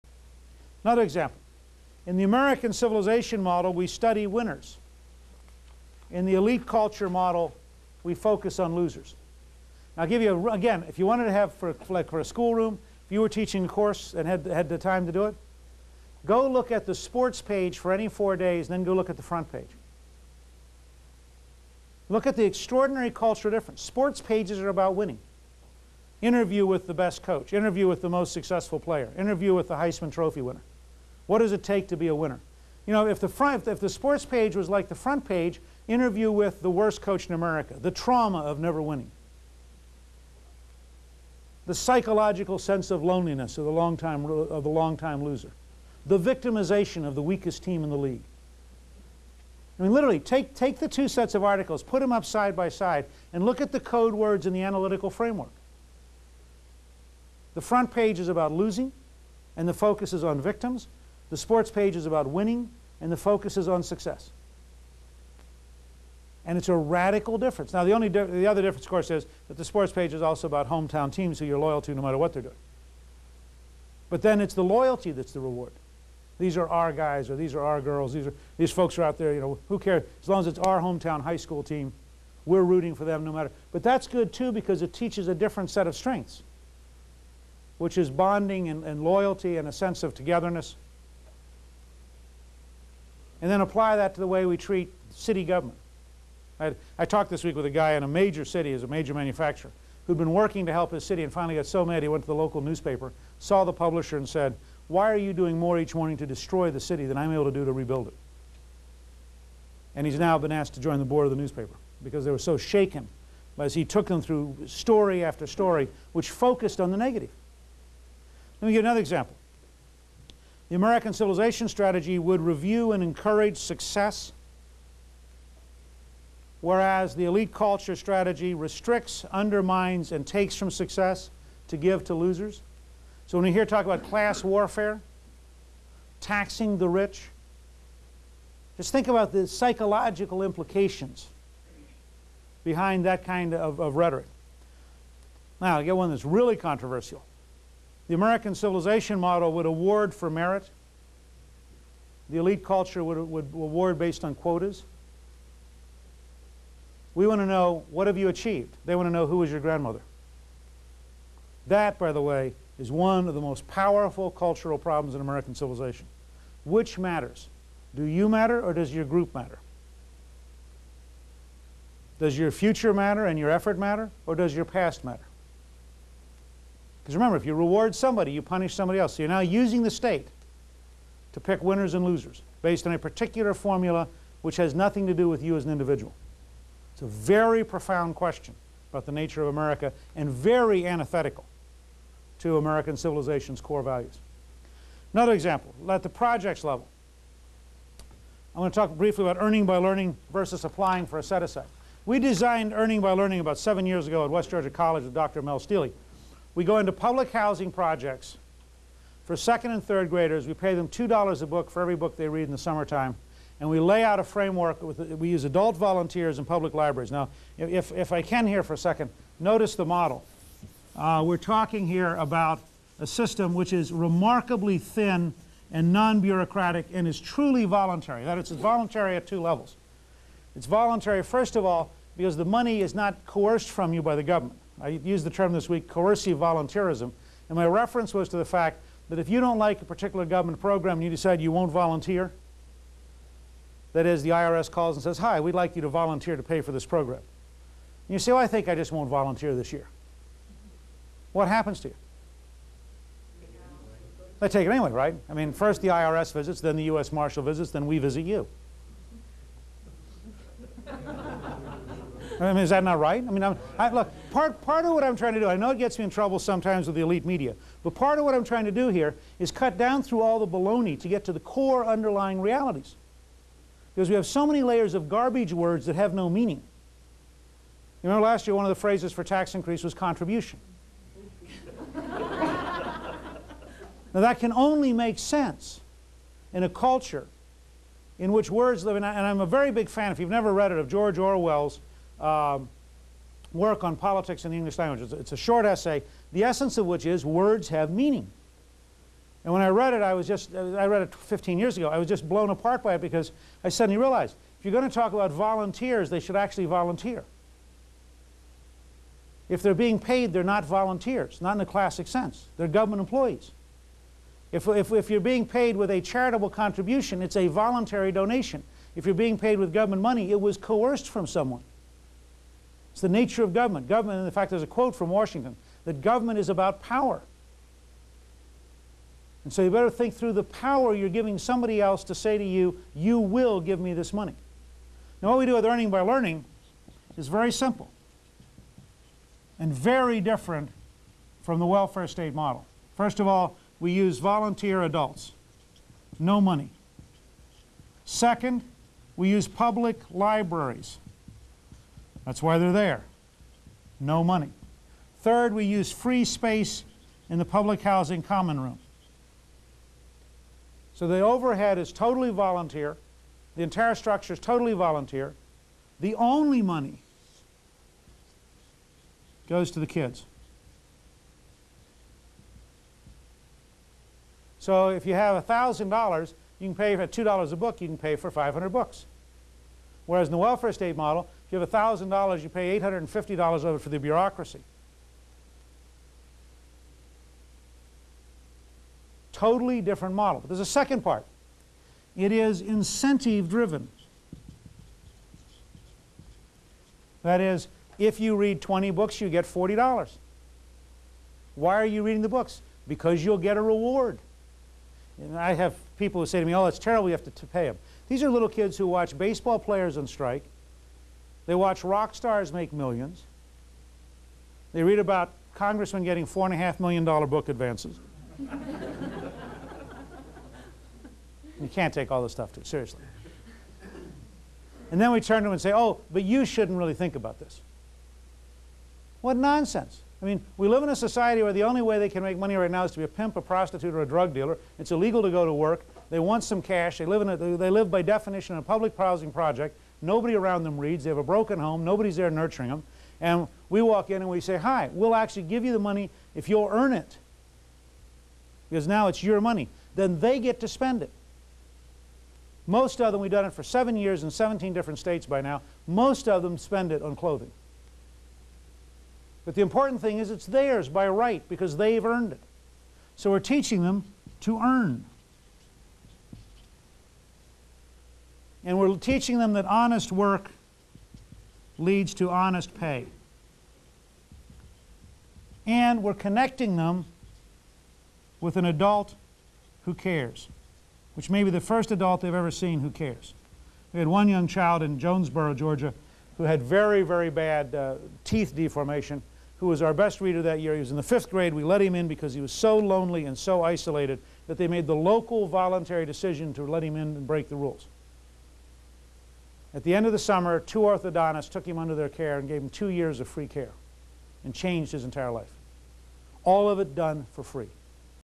RAC03b.mpg
Renewing American Civilization Course - Lecture Three -- January 21st, 1995 -- Pillar Two, Personal Strength (Part 02 of 13)